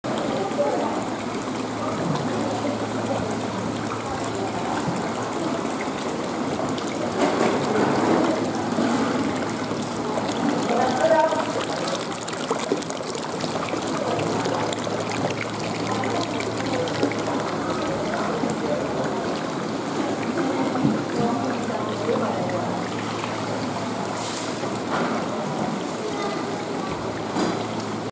大きな紀州産天然本クエが何本も
年末年始用のようです。